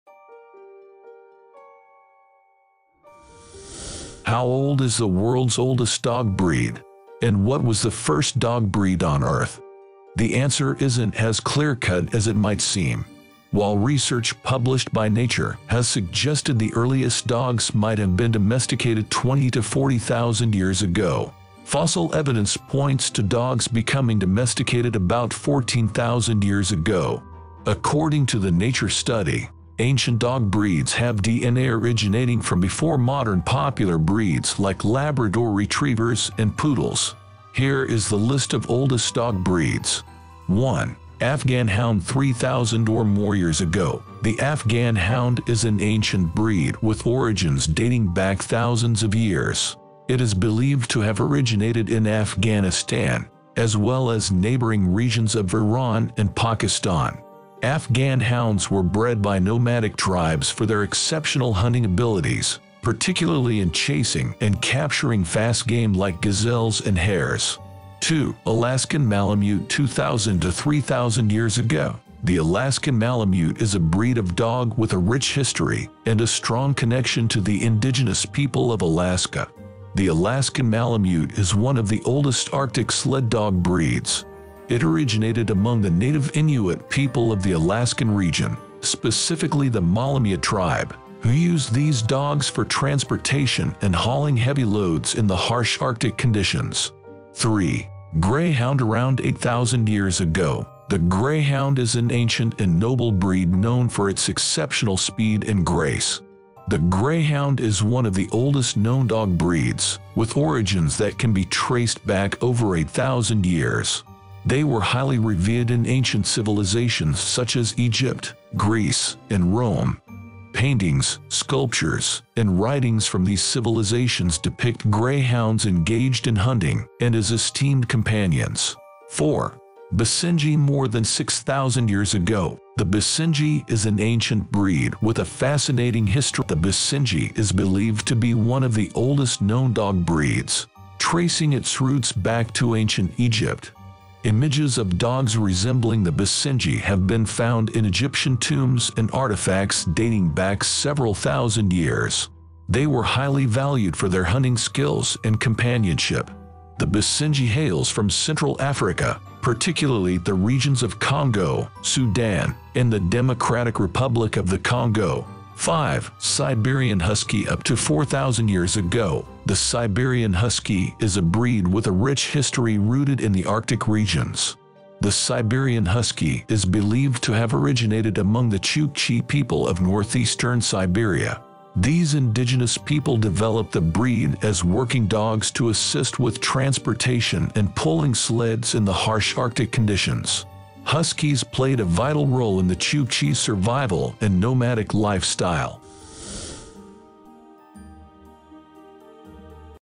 World’s Oldest DOG Breeds - Oldest Dog Breeds
Oldest dog breeds refer to the breeds that have existed for thousands of years and have retained their distinct characteristics and traits over time. These breeds have a long history and are often considered living links to the past. While the exact origins and timelines of dog breeds are sometimes uncertain, several breeds are commonly recognized as among the oldest

1. Afghan Hound (3,000 Or More Years Ago)
2. Alaskan Malamute (2,000 To 3,000 Years Ago)
3. Greyhound (Around 8,000 Years Ago)
4. Basenji (More Than 6,000 Years Ago)
5. Siberian Husky (Up To 4,000 Years Ago)